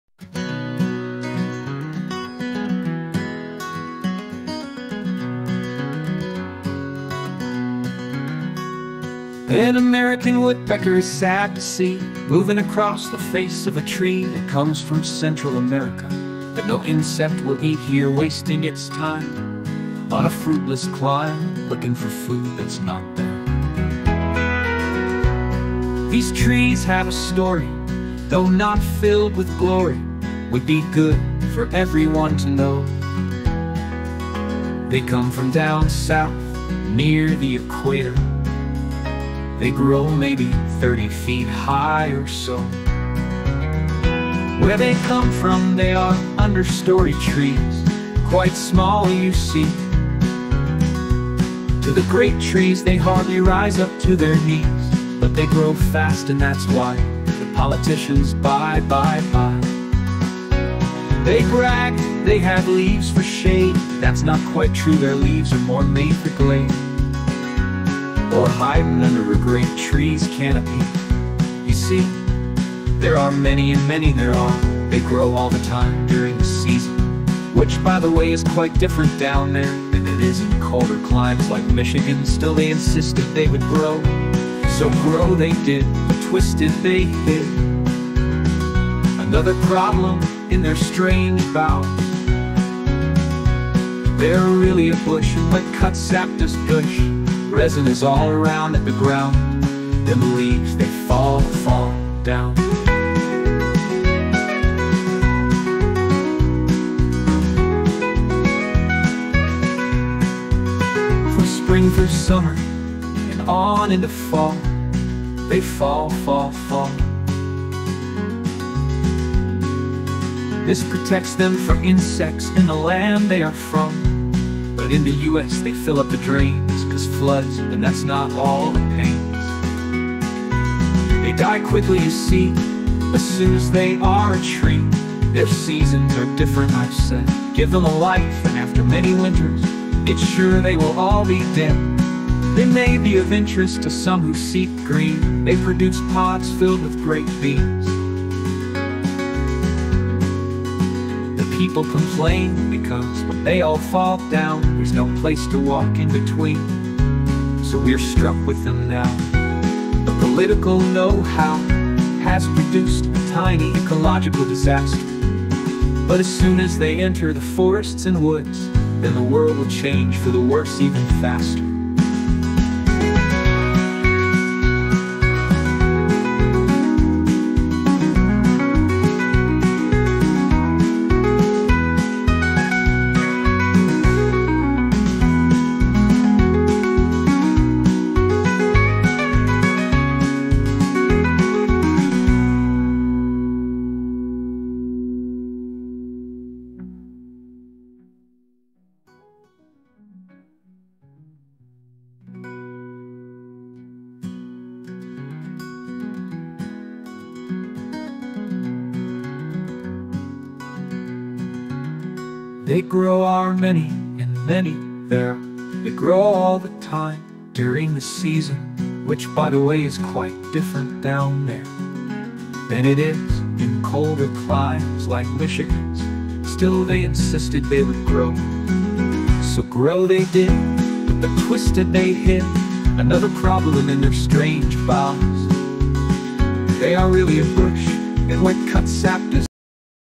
Bean Trees
An American Woodpecker is sad to see
Moving across the face of a tree
That comes from Central America
That no insect will eat here
Wasting its time
On a fruitless climb
Looking for food that’s not there

These trees have a story
Though not filled with glory
Would be good for everyone to know
They come from down south
Near the equator
They grow maybe thirty feet high or so
Where they come from they are understory trees
Quite small you see
To the great trees they hardly rise up to their knees
But they grow fast and that’s why
The politicians buy, buy, buy
They bragged they have leaves for shade
That’s not quite true their leaves are more made for glade
Or hiding under a great tree’s canopy
You see, there are many and many there are
They grow all the time during the season
Which, by the way is quite different down there
Than it is in colder climes like Michigan’s
Still they insisted they would grow
So grow they did but twisted they hid
Another problem in their strange boughs
They are really a bush and went cut sap does gush
Resin is all around at the ground
Then the leaves, they fall, fall, down
From Spring through Summer and on into fall
They fall, fall, fall
This protects them from insects in the land they are from
But in the US they fill up the drains
Cause floods and that’s not all the pains
They die quickly you see, as soon as they are a tree
Their seasons are different I’ve said
Give them a life and after many winters
It’s sure they will all be dead
They may be of interest to some who seek green
They produce pods filled with great beans
The people complain because when they all fall down
There’s no place to walk in between
So we are struck with them now
The political know how
Has produced a tiny ecological disaster
But as soon as they enter the forests and woods
Then the world will change for the worse even faster